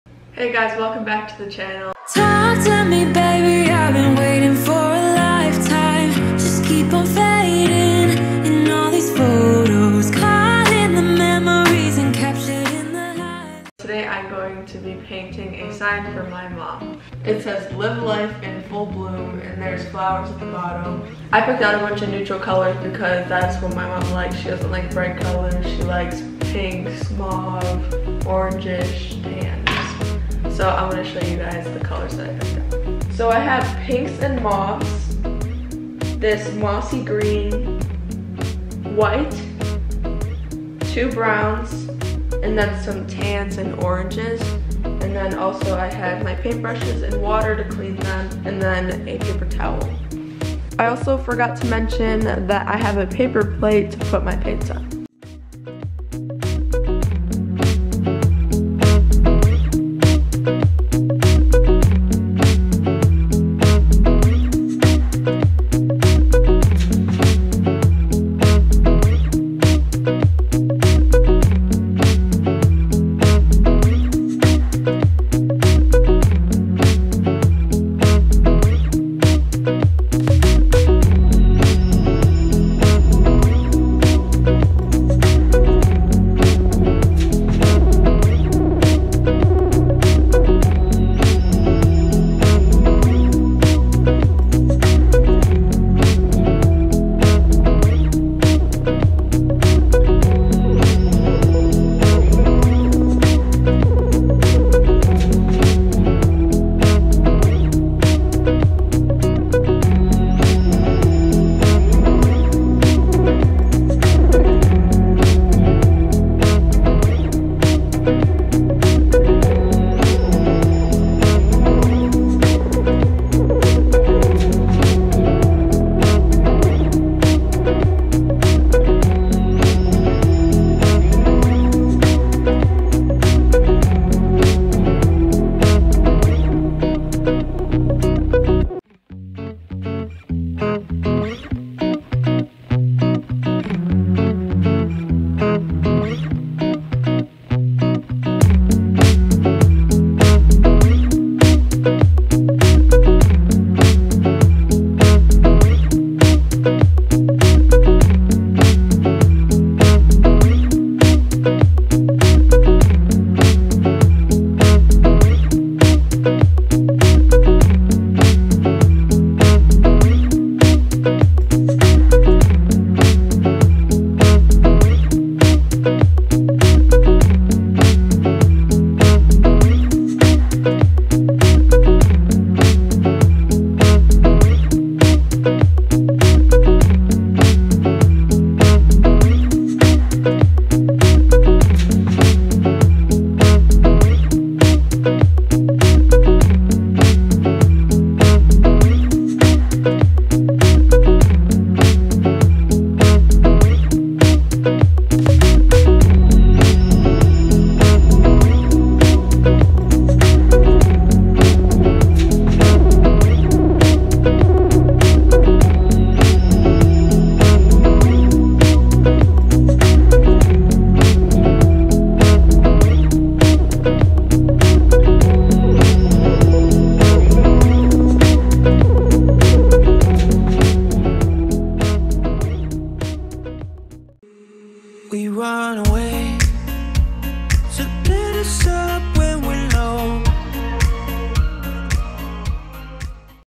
I painted a sign for EASTER! | Satisfying
I painted this sign for my mom and so we have some Easter decor! Thank you guys for watching!